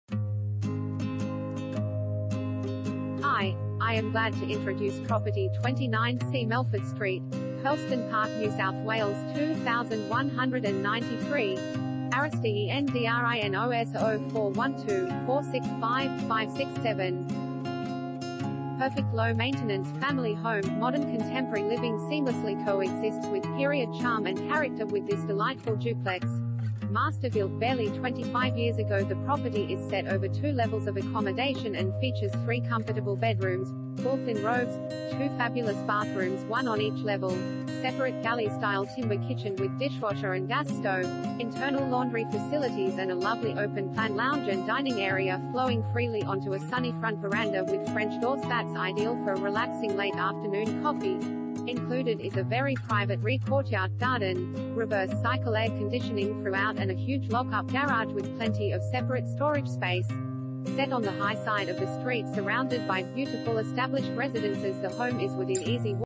29C Melford Street, Hurlstone Park NSW 2193 | AuPropertyReport.Com
29C Melford Street, Hurlstone Park NSW 2193
Property Type: house
Bedrooms: 3
Bathrooms: 3
Car Space: 2
ARIS DENDRINOS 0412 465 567